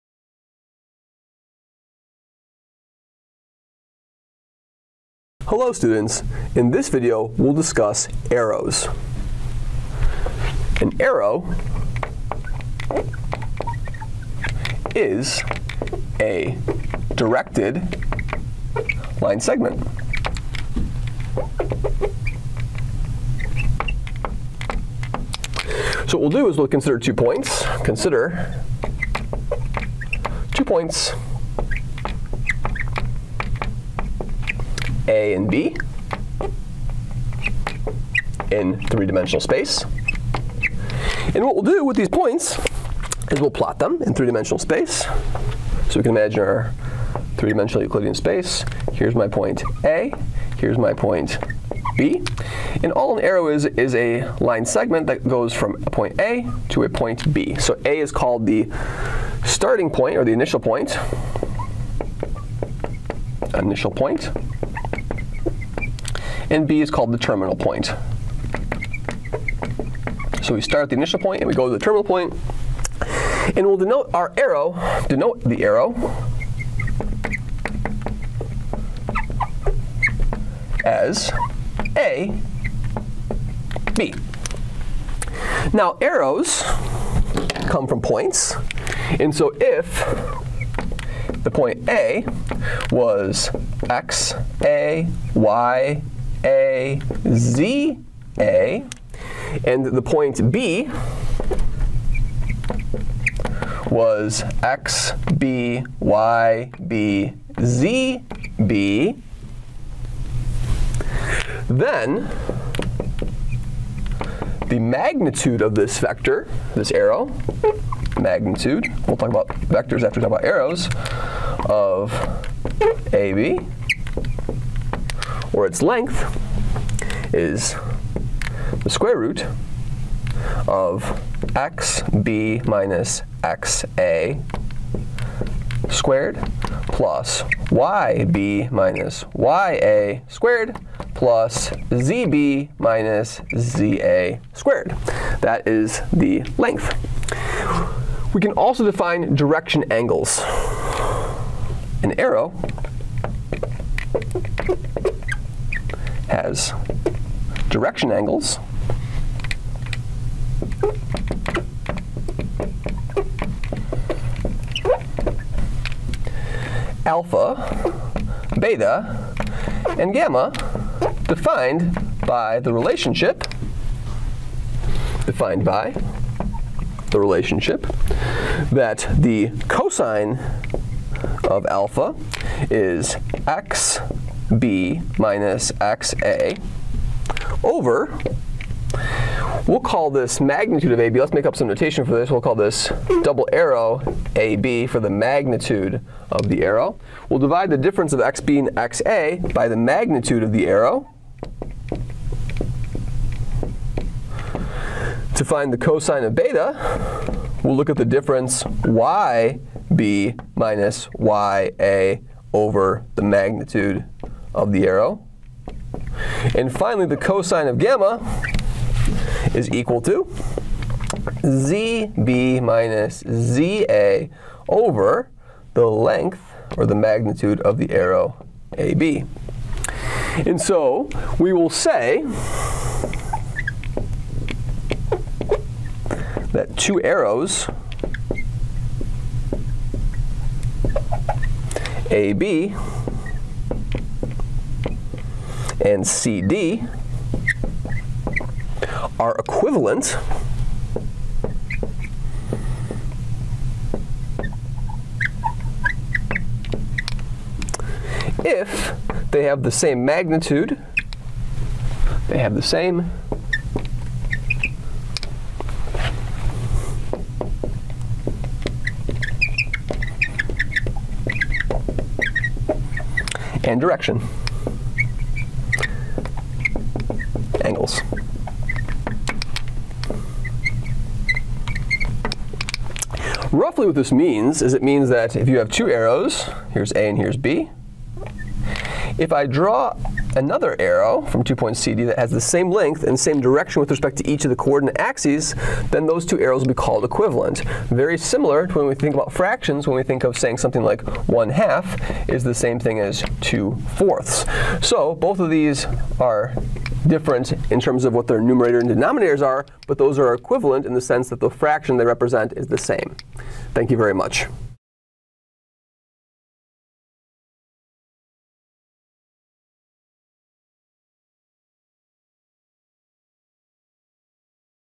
Arrows
We define arrows, which are directed line segments. Arrows are used the define vectors.